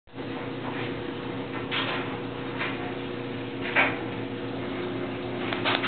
Cat Clapping
This is our cat Myles, we taught him to clap!